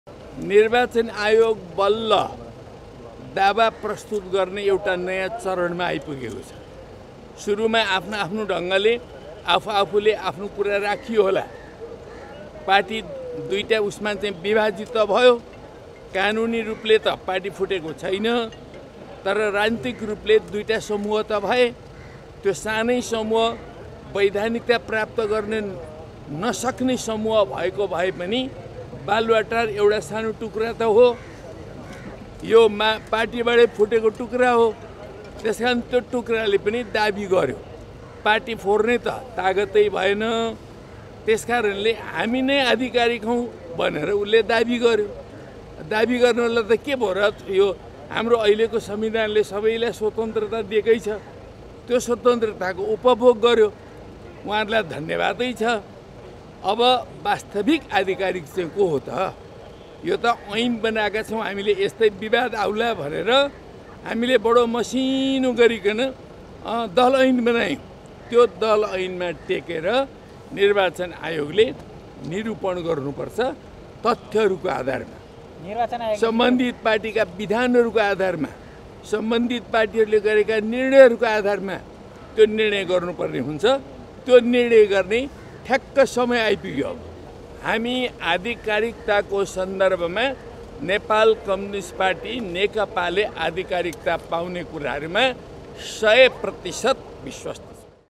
२७५ केन्द्रीय सदस्यको हस्ताक्षर सहित प्रचण्ड-माधव समूहको नेकपामा दाबी, के छ ऐनमा व्यवस्था?
२७५ केन्द्रीय सदस्यको हस्ताक्षर सहित प्रचण्ड-माधव समूहको नेकपामा दाबी, के छ ऐनमा व्यवस्था?